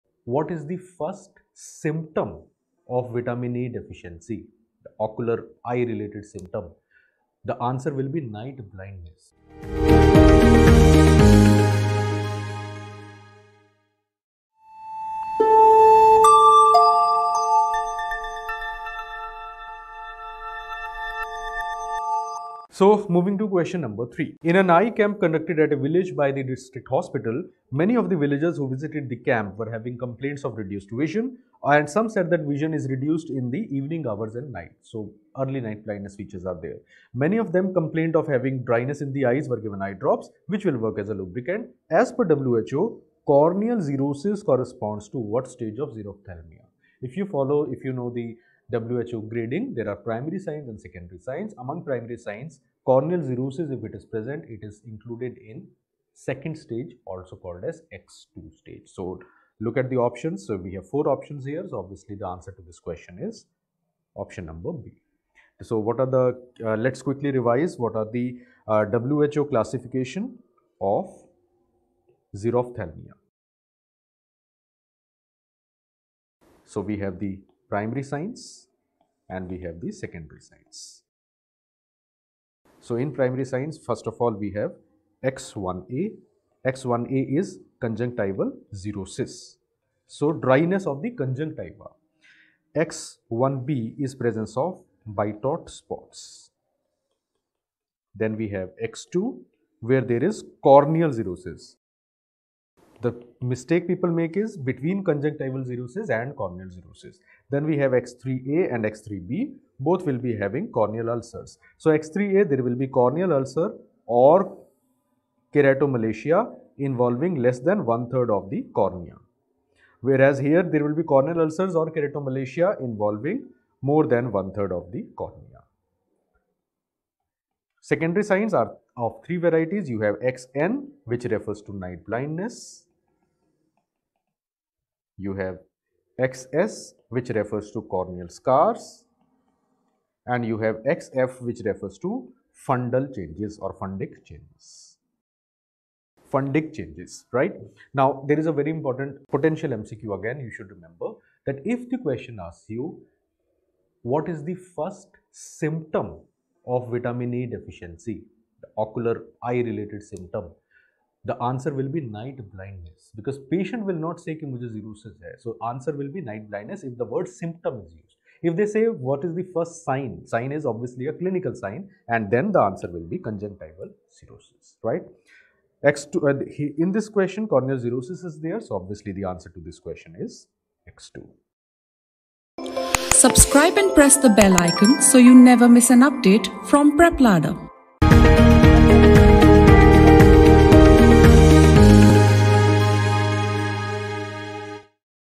Xerophthalmia by Dr. Sandeep Sharma - PrepLadder [NEET SS]
Join Dr. Sandeep Sharma, faculty of Pediatrics on PrepLadder in this amazing session where he explains the topic Xerophthalmia .